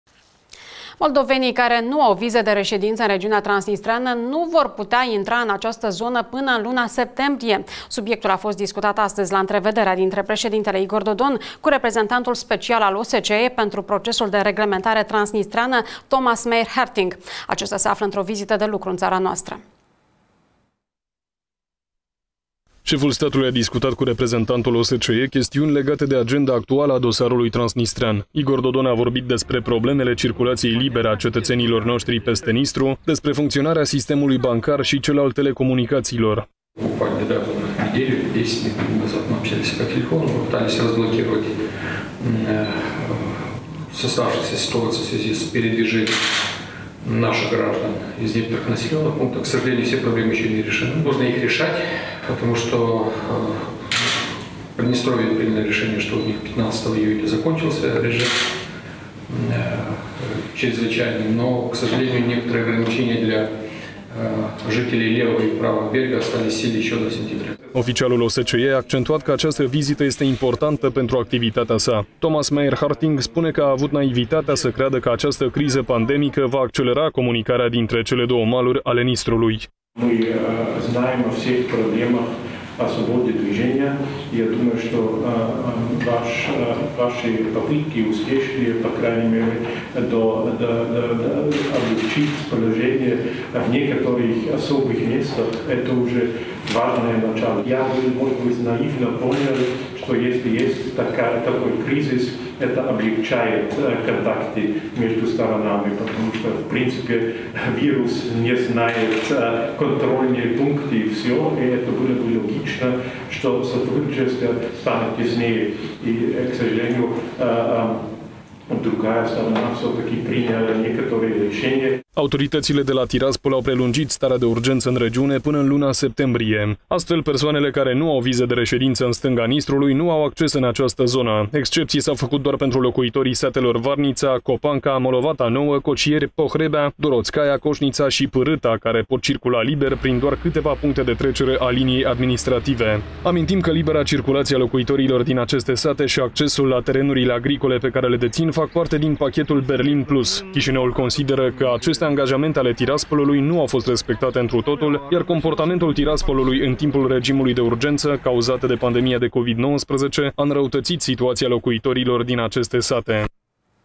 LIBERA CIRCULAȚIE, DISCUTATĂ CU OSCE
Moldovenii, care nu au viză de reședință în regiunea transnistreană, nu vor putea intra în această zonă până în luna septembrie. Subiectul a fost discutat astăzi, la întrevederea dintre președintele Igor Dodon cu reprezentantul special al OSCE pentru procesul de reglementare transnistreană, Thomas Mayr-Harting. Acesta se află într-o vizită de lucru în țara noastră.